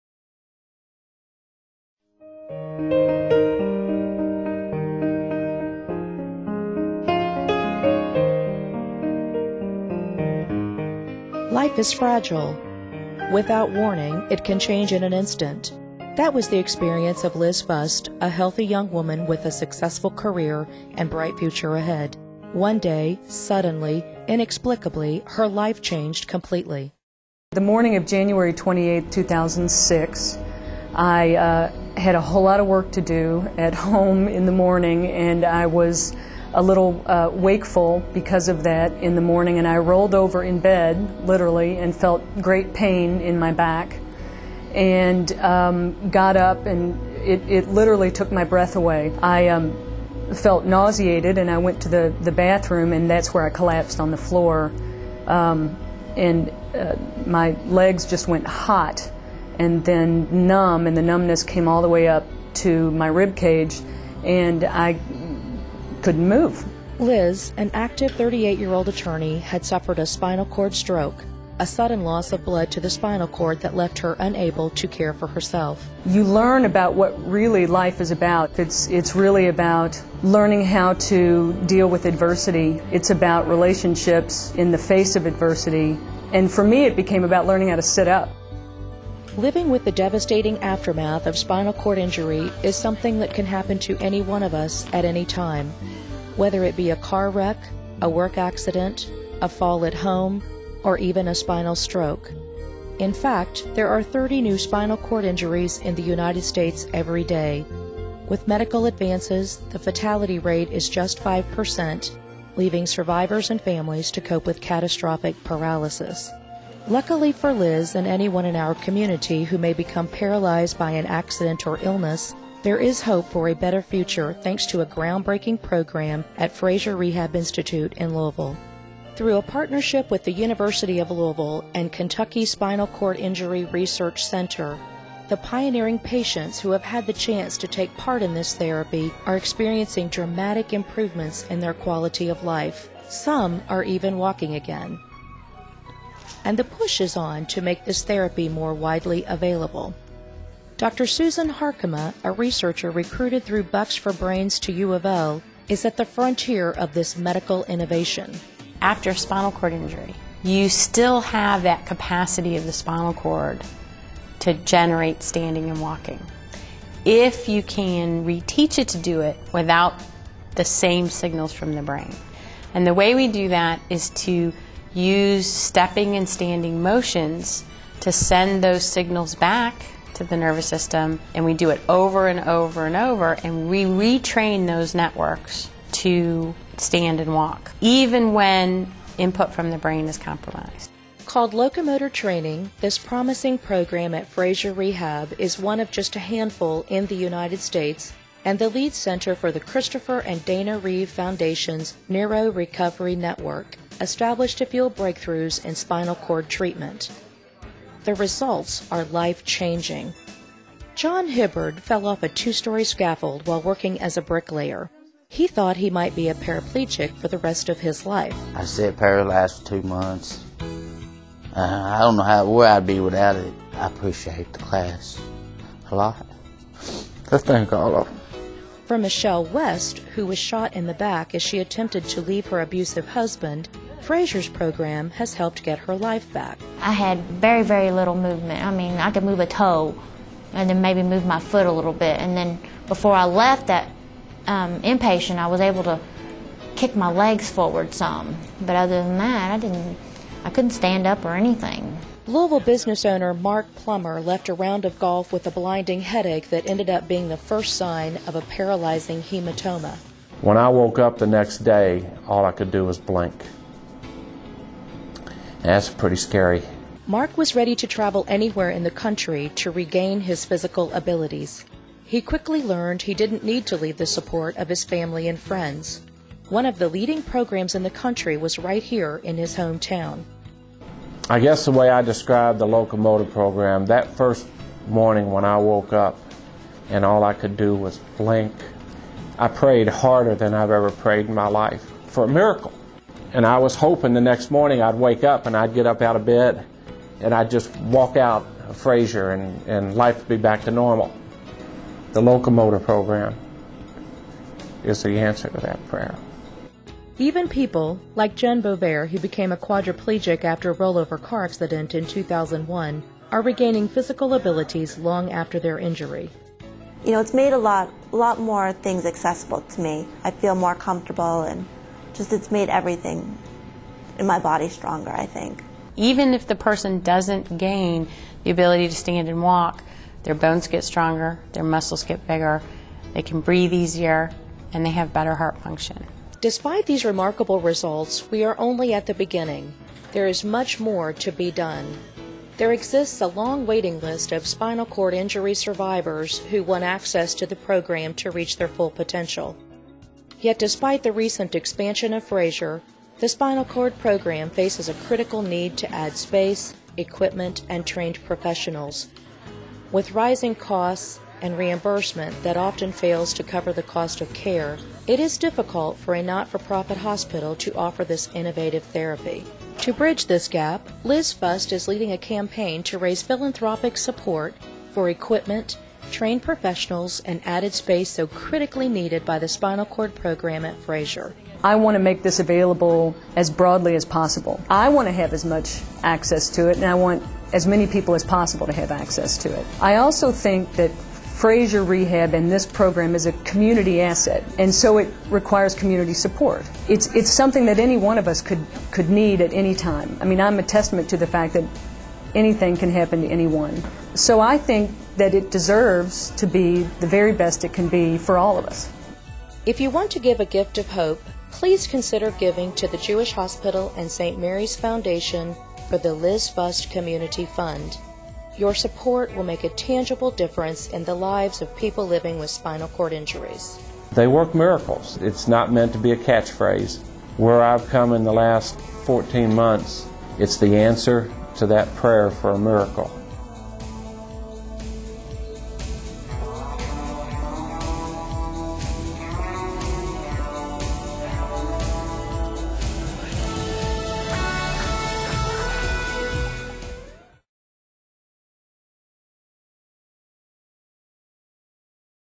The Liz Fust Community Fund
Hope for a Brighter Future for People with Spinal Cord Injuries For Liz and thousands of people living with the devastating aftermath of spinal cord injury, there is hope for a better future thanks to a new rehabilitation program at Frazier Rehab Institute. Through a partnership with the University of Louisville and its Kentucky Spinal Cord Injury Research Center, people who have been paralyzed by injury or accident are regaining physical abilities that dramatically improve their quality of life. And some are even walking again.